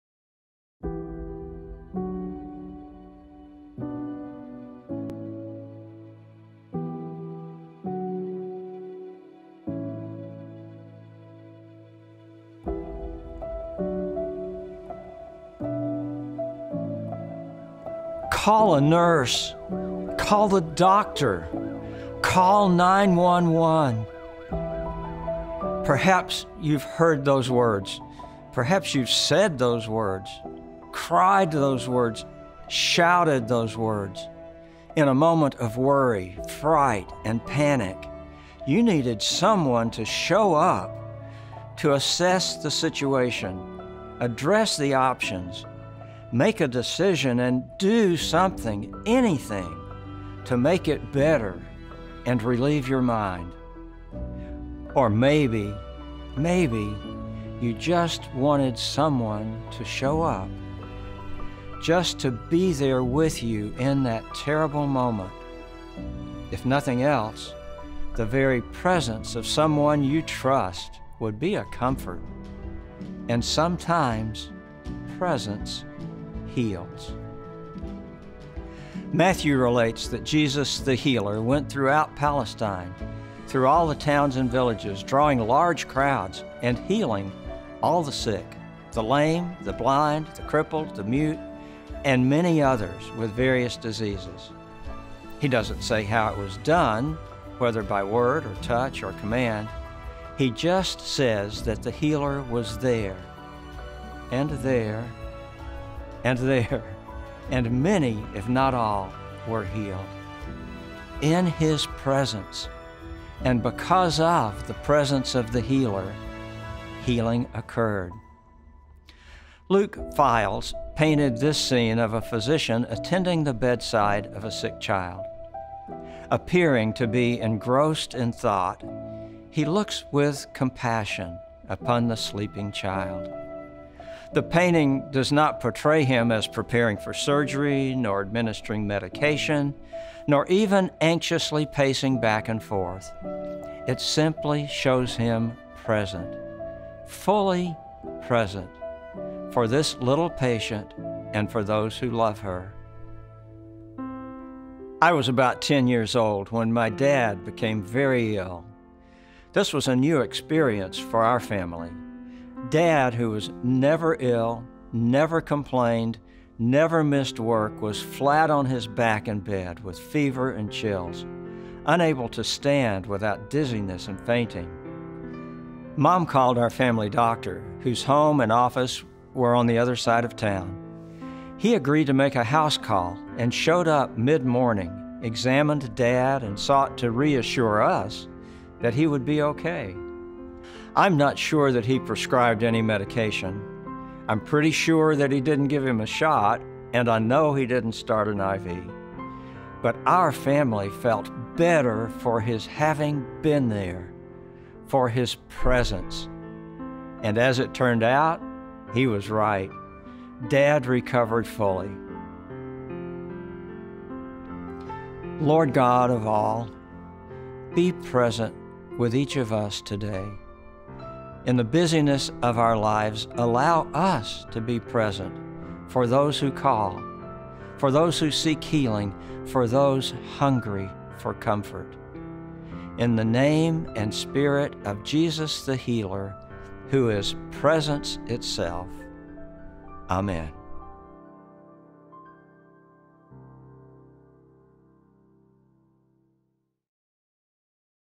Week of Spiritual Renewal - The Healer’s Presence
Do you remember a time when you felt the comfort of someone else’s presence? Perhaps it was the love of a parent when you were sick, or a visit by a friend during a time of loss. Today’s message tells us to go out and share that same comfort by being present for somebody who needs it most.

God is always involved in our lives because He cares for us deeply. He is constantly guiding our path and teaching us His wisdom. When we are open to receive it, we can sense His healing presence with us.

Today, I pray that you experience His healing presence in your life, and that you reflect this to others by being there when they need you most.